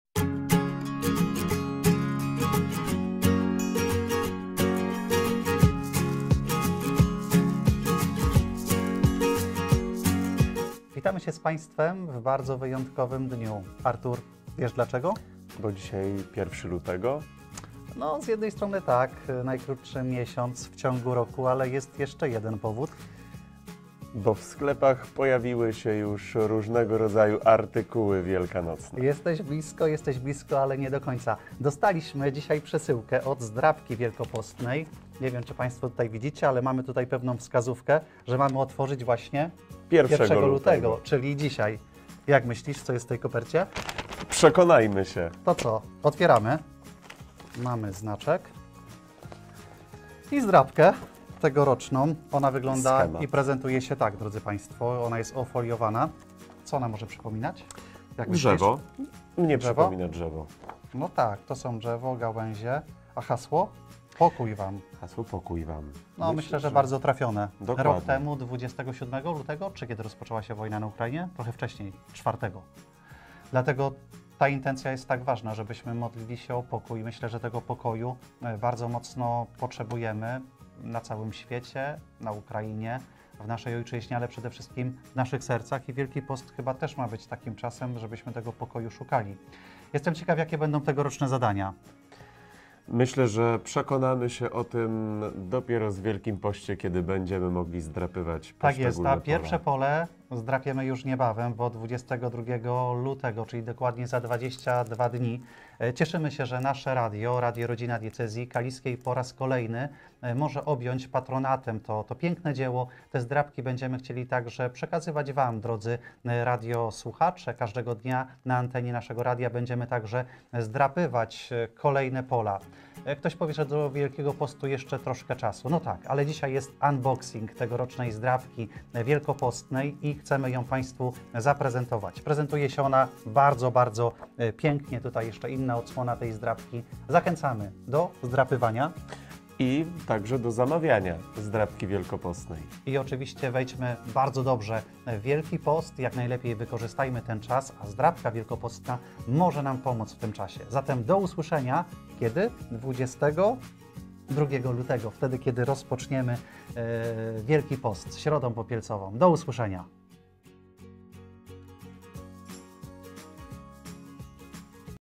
UNBOXING - ZDRAPKA WIELKOPOSTNA 2023
W Radiu Rodzina Diecezji Kaliskiej otwieraliśmy paczkę z